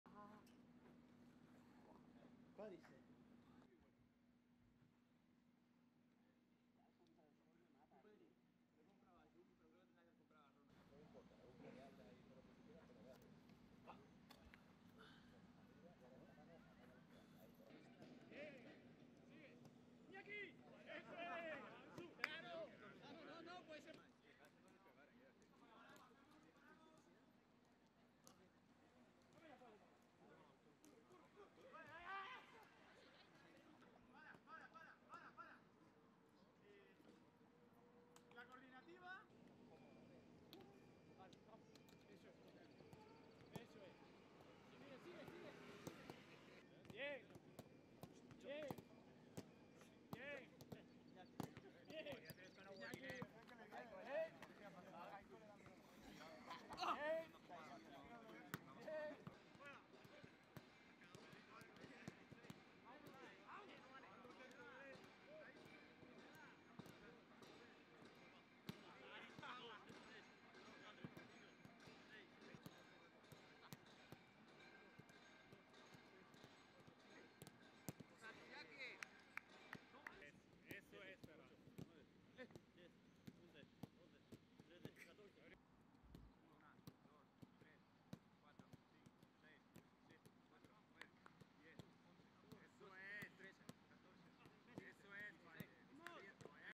Entrenamiento del BARÇA: Iñaki Peña y Astralaga entrenan con el equipo tras la lesión de Ter Stegen
El FC Barcelona ha entrenado esta mañana en la Ciutat Esportiva tras la victoria de ayer en La Cerámica ante el Villarreal (1-5). A pesar del buen resultado, la lesión de Ter Stegen dejó un sabor amargo. Esta mañana el club azulgrana ha asegurado en un comunicado médico que el guardameta pasará por el quirófano esta misma tarde. El portero alemán sufre una rotura completa del tendón rotuliano de la rodilla derecha sufrida ayer al final de la primera parte del Villarreal-Barça. El cancerbero no quiere perder tiempo en su recuperación. El club azulgrana señala que facilitará otro comunicado médico cuando finalice la intervención.

Hansi Flick no dispondrá de Ter Stegen en lo que queda de temporada, pero el técnico alemán cuenta con Iñaki Peña y Ander Astralaga, portero del Barça Atlètic.